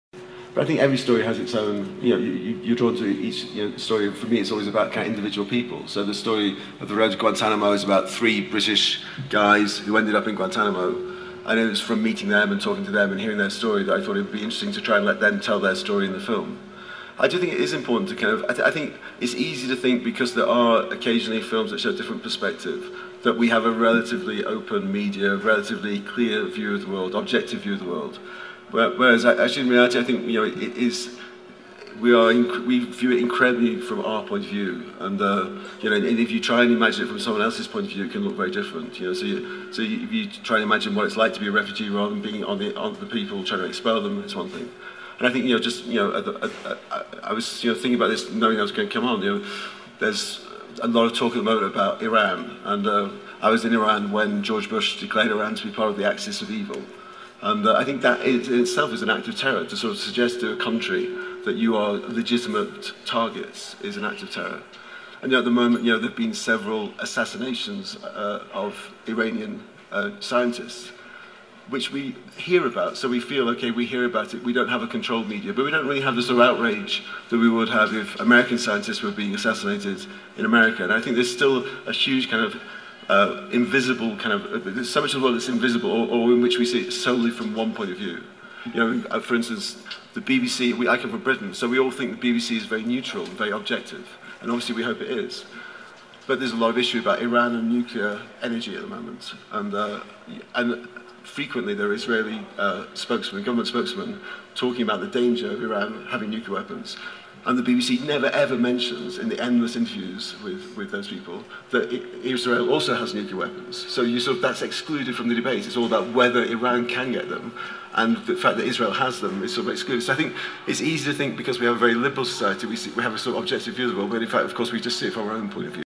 Directors Panel Telluride Film Festival, Ben Affleck on "Argo"
Annette Insdorf moderates panel at Telluride with directors of "Argo," "The Gatekeepers," "The Attack," "The Art of Killing," "Everyday."